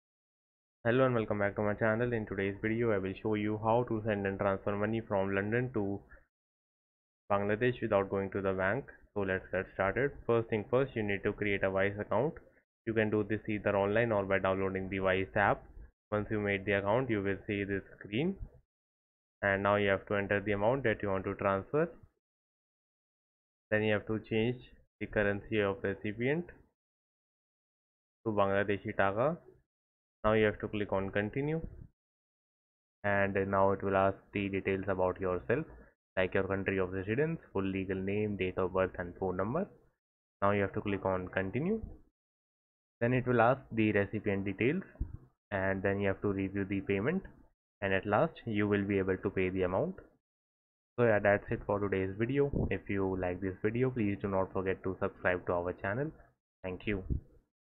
✅ How To Send Money From London to Azerbaijan Without Going To Bank (Full Guide)
Learn how to send money from London to Azerbaijan without visiting a bank with this detailed guide. Follow the step-by-step instructions for a secure and easy transfer.

Key Learning Points:
• Step-by-step guide for sending money from London to Azerbaijan
• Understanding online transfer options
• Important tips for secure money transfers